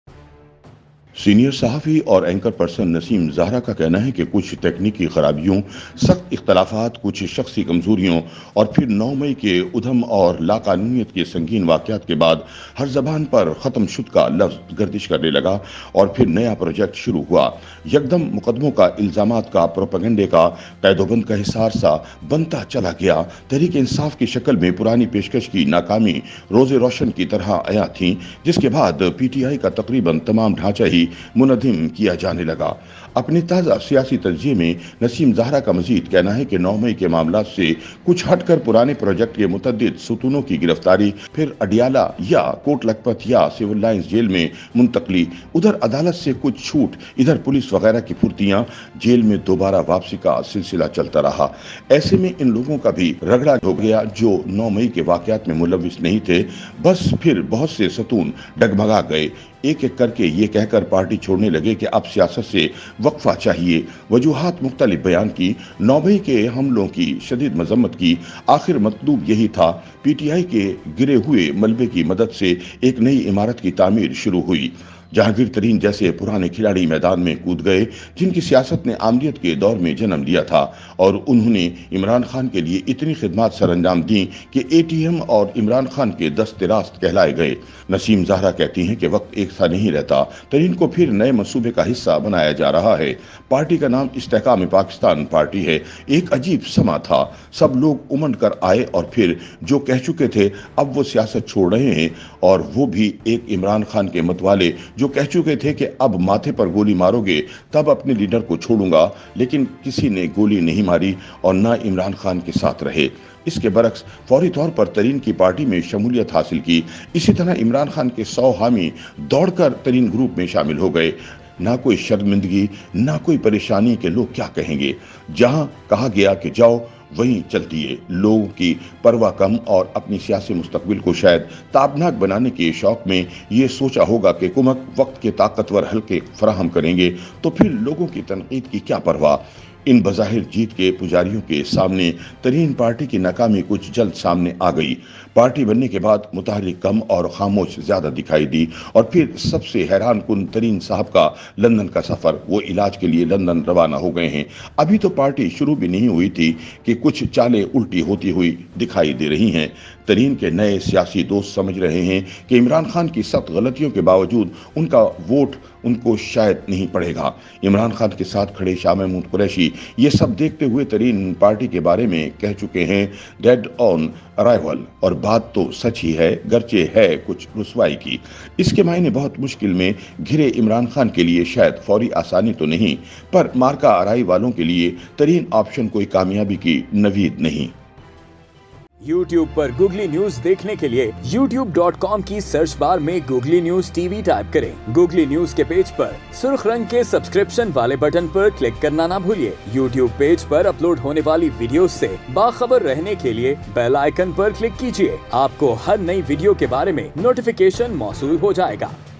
Kya Jahangir Tareen Imran Khan Ko Shikast Dai Sakain Gai? : Naseem Zehra | Googly News TV
Kya Jahangir Tareen Imran Khan Ko Shikast Dai Sakain Gai? : Naseem Zehra